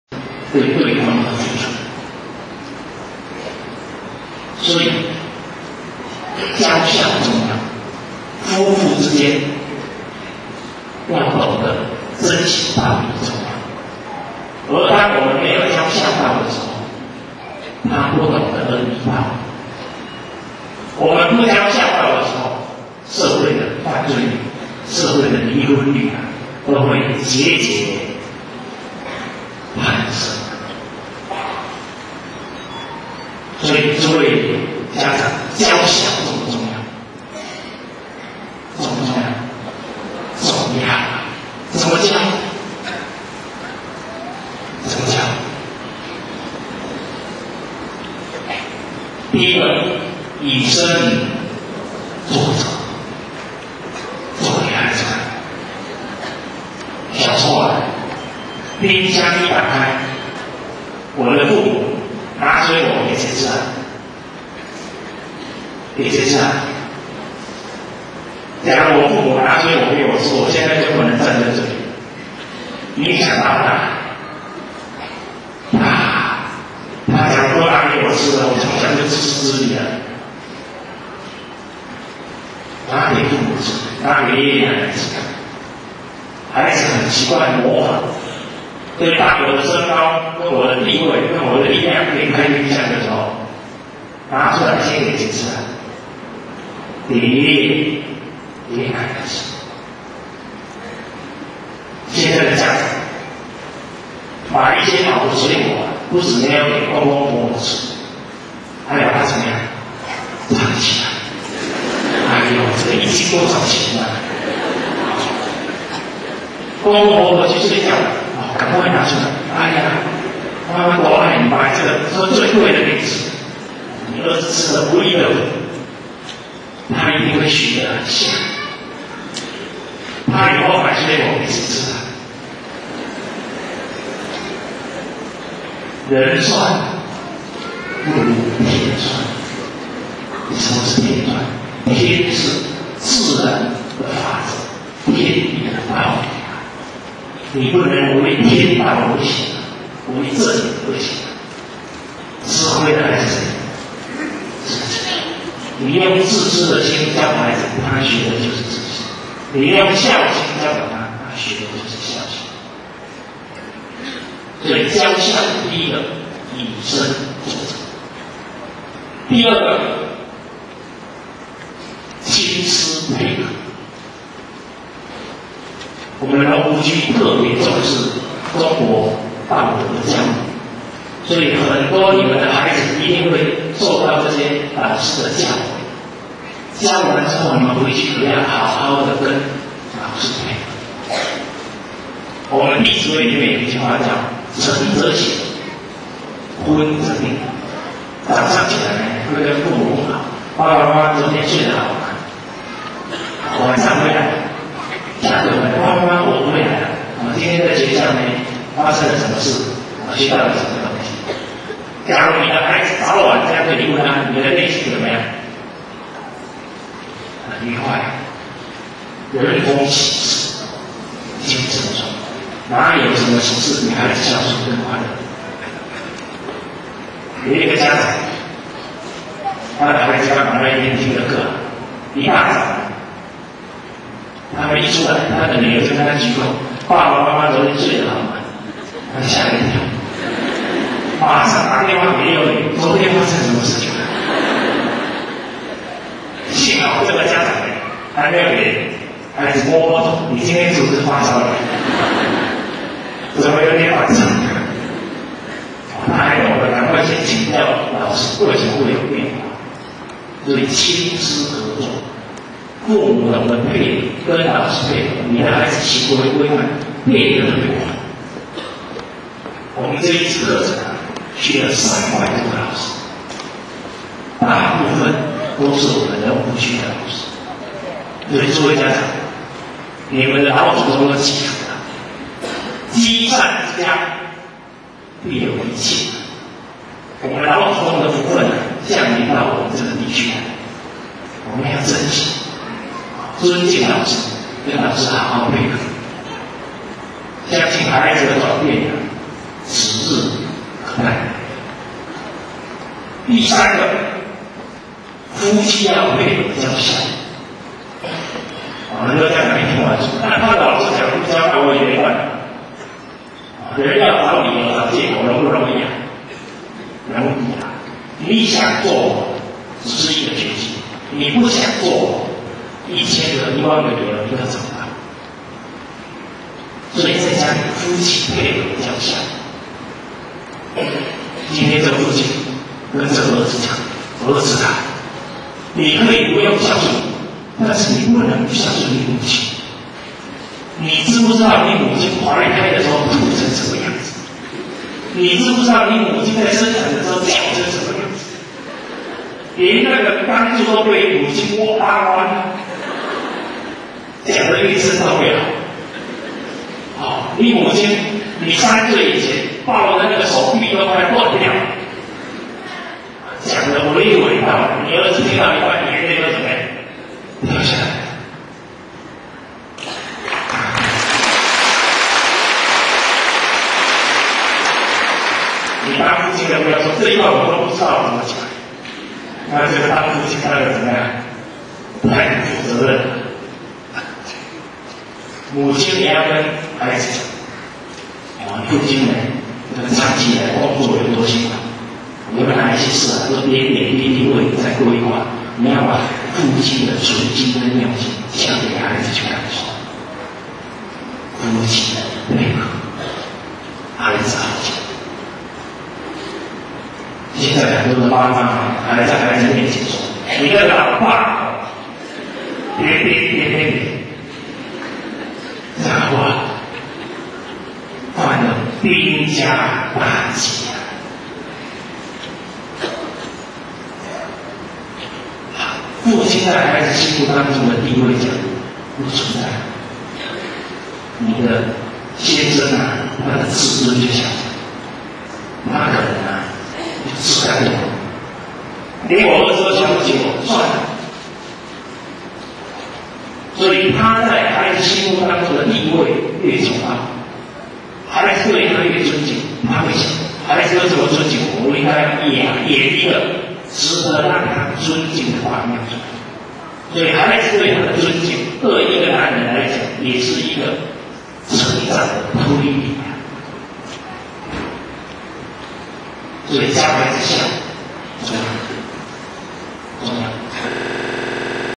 蔡礼旭老师讲: 夫妇珍惜恩义 身教孩子孝道
蔡礼旭老师主讲：如何落实德行教育演讲会 (下篇) 2004年11月7

夫妇之间要懂得珍惜恩义道义。而当我们没有教孝道的时候，他不懂得恩义道义。我们不教孝道的时候，社会的犯罪率、社会的离婚率都会节节攀升。诸位家长，教孝道重不重要？重要啊。

怎么教孩子孝道？

第一个：「以身作则」。做给孩子看。小时候冰箱一打开，我的父母拿水果给谁吃啊，给谁吃？假如我父母拿水果给我吃，我现在就不能站在这里了。父母影响大不大？大啊。她假如都拿给我吃了，我从小很可能就自私自利了。拿给父母吃，拿给爷爷奶奶吃，孩子很习惯模仿。当我的身高跟我的力量可以开冰箱的时候，拿出来先给谁吃啊？当然给爷爷给奶奶吃。现在的家长买一些好的水果，不是先给公公婆婆吃，还把它藏起来，「哎哟」，这个一斤多少钱呐，公公婆婆去睡觉了，赶快出来。哎哟，妈妈多爱你，买这个最贵的给你吃。」你儿子吃得不亦乐乎，他一定会学得很像，他以后买水果给谁吃啊？人算不如天算，什么是天算？天是自然的法则，天然的道理，你不能违背天道而行，违正理而行。吃亏的是谁？自己啊。你用自私的心教孩子，他学的就是自私；你用孝心教导他，他学的就是孝心。所以教孝道第一个是以身作则。

第二个：「亲师配合」。古时候，当孩子第一天上学，父母第一句话：「在学校里要尊敬老师听老师的话。」学生放学回家之前，老师会对学生说：「回到家里要孝顺父母，帮助父母做家务。」我们龙湖区特别重视中国道德的教育，你们的孩子一定会受到这些老师的教诲，教完之后你们回去可要好好的跟老师配合。《弟子规》里面有一句话叫「晨则省，昏则定。」早上起来要跟父母问好，「爸爸妈妈，昨日睡的得好吗？」晚上回来，「爸爸，妈妈我回来了」，还讲今天在学校发生了什么事，学到了什么东西。假如你的孩子早晚会跟你问安，你内心会怎么样？很愉快。人逢喜事精神爽，哪有什么喜事比孩子孝顺更快乐的。有一个家长，他的孩子刚好那一天听了课，一大早还没出门，他的女儿就跟他鞠躬问候：「爸爸妈妈，昨日睡得好吗？」他吓了一跳，马上打电话给幼儿园，「昨天发生了什么事情？」幸好这个家长还没有摸摸孩子的头，说你今天是不是发烧了，怎么会这么讲？他还懂得先请教老师为什么会有变化。所以亲师合作，父母懂得跟老师配合，你的孩子行为规范就变得特别快。我们这一次培训班去了三百多个老师，大部份都是我们龙湖区的老师。所以诸位家长，你们的祖宗都积福了。积善之家，必有余庆。我们老祖宗的福份降临到我们这个地区来，我们要 珍诊惜。尊敬老师，跟老师好好配合，相信孩子的转变指日可待。

第三个：「夫妻配合教孝道」。很多家长一听完便说，那他的老师不教我也没办法。人要找理由找借口，容不容易啊？容易啊。你想做只是一个决心，你不想做，一千个一万个理由你都找得到，所以在家里夫妻要配合教孝道。今天这个父亲跟女儿讲：「孩子啊，你可以不用孝顺我，但是你不能不孝顺你母亲，你知道不知道你母亲怀胎的时候吐成什么样子，你知不知道你母亲在生产的时候叫成什么样子， 连那个钢柱都被母亲拉弯了，痛得死去活来！」讲得愈生动愈好，「你母亲，你三岁以前抱得那个手臂都快要断掉了。」娓娓道来，你儿子听到一半眼泪都掉下来了。你当父亲的不要说这一些我都不知道，那你这个当父亲的，当得怎么样？太不负责任了！母亲也要跟孩子讲，你父亲长期以来工作有多辛劳，很多事都点点滴滴为你在 规划，你要把父亲的存心跟用心，讲给孩子去感受。夫妻配合，教好孩子孝行。现在很多的妈妈还在孩子面前说：「你的爸爸怎样. 」，这样好不好？犯了兵家大忌。父亲在孩子心目中地位假如不存在了，你先生的自尊就下降了，他可能就自甘堕落，「连我儿子都瞧不起我，算了」。假如他在孩子心目中的地位愈祟高，孩子对他愈尊敬，他会想孩子都这么尊敬我，我应该要演一个值得让他尊敬的榜样出来。孩子对父亲的尊敬，对一个男人来讲，也是一个成长的动力啊，所以教孩子孝道重要。